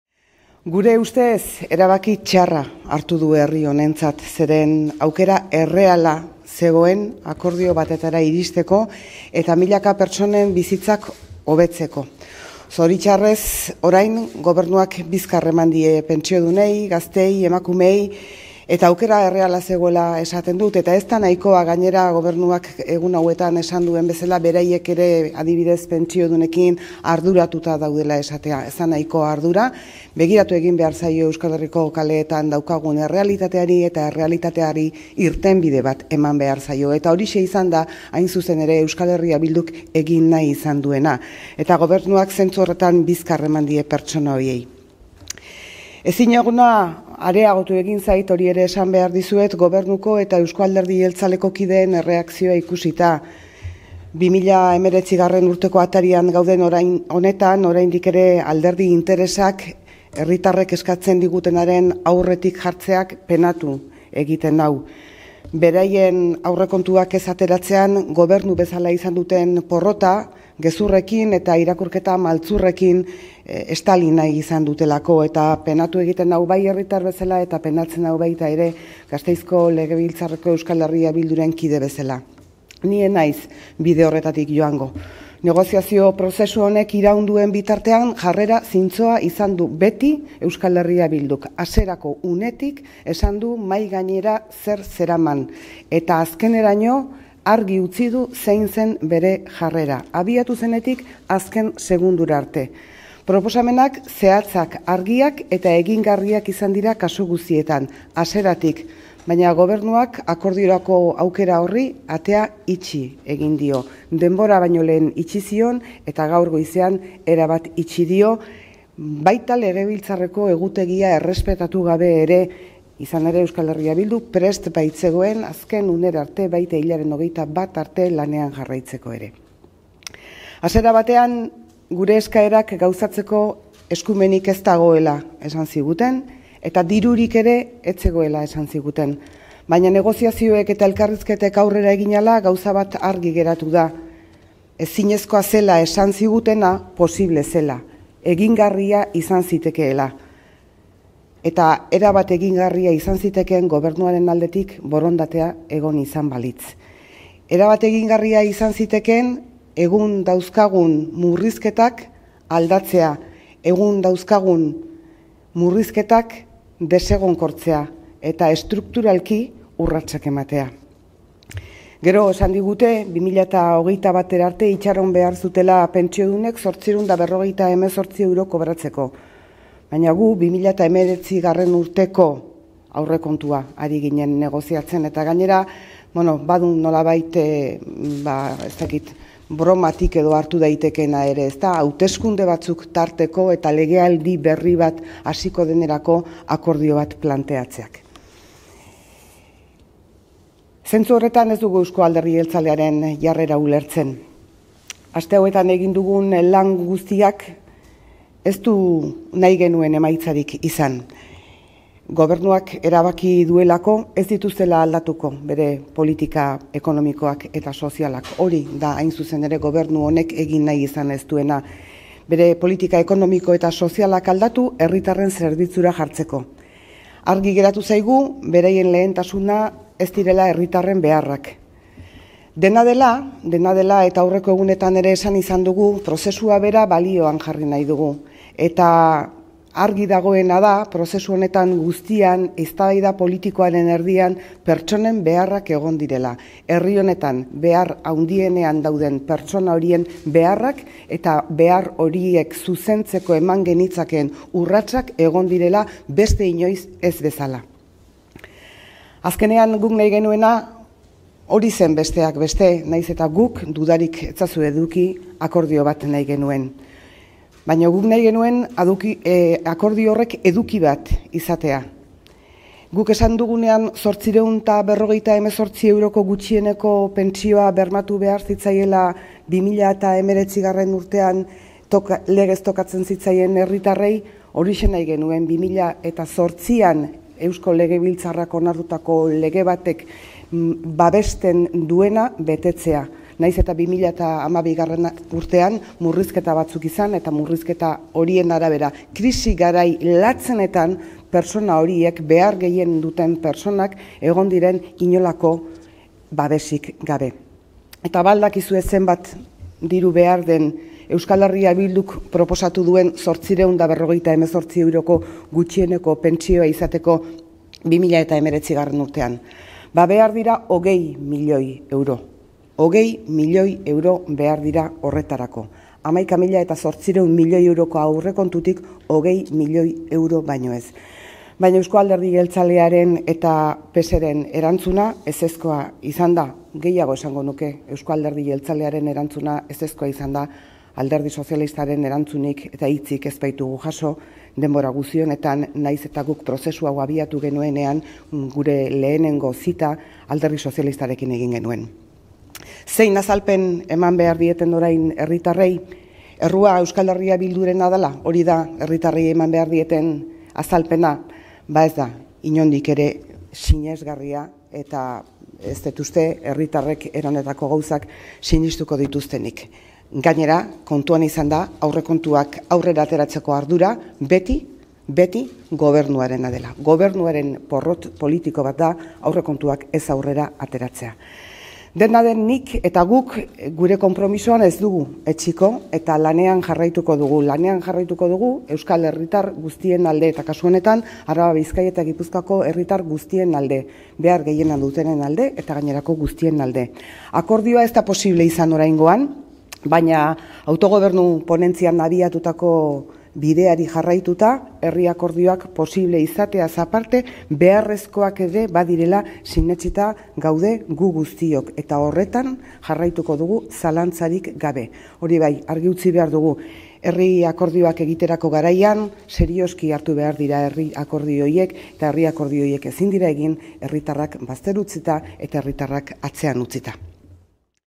Maddalen Iriarte: "Penatua nago gobernuak izan duen jarrerarekin"
📽 @MaddalenIriarte-ren adierazpenak gaur arratsaldeko agerraldian: "Nahi genuen 858€ko gutxieneko pentsioa bermatu 2019an, herritarrei legez tokatzen zaiena. Eta, badakizue zenbat diru behar den 858€ko gutxieneko pentsioa izateko 2019an? 20 milioi €. 11.800 milioi €ko aurrekontutik, 20 milioi. Erantzuna ezezkoa izan da".